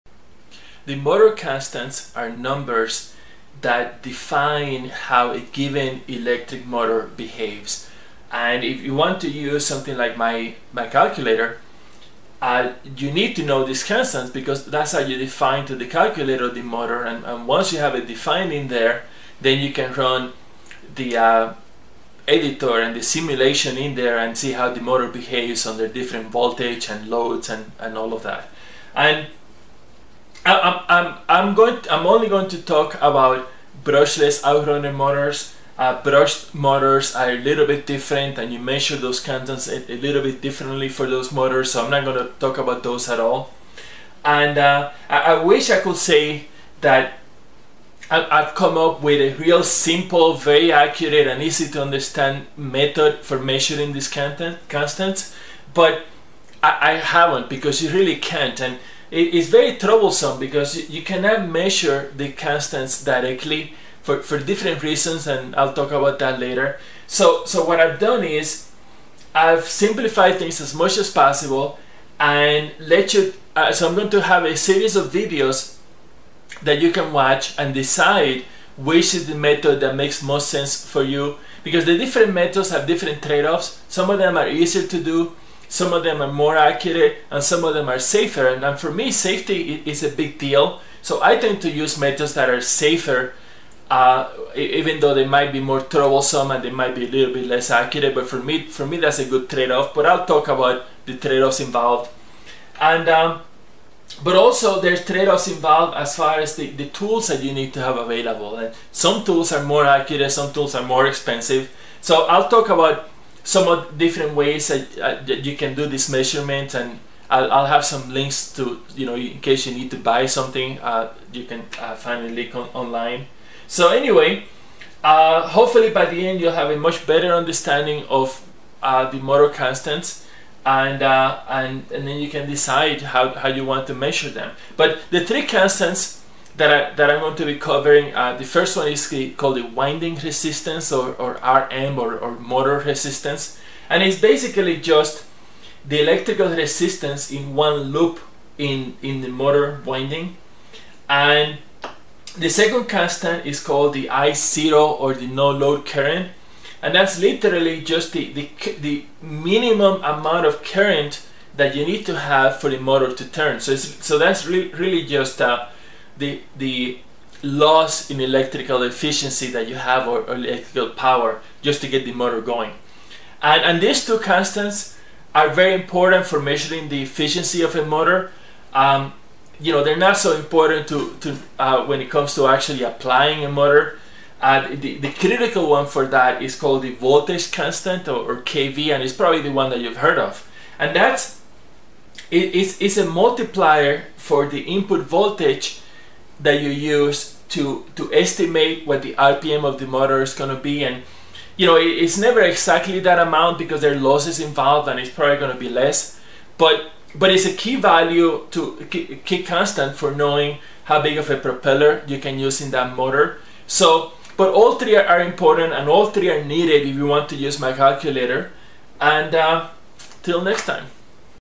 Measuring Motor Constants: Introduction
Introduction to measuring the electric brushless outrunner motor constants Rm, I0, and Kv.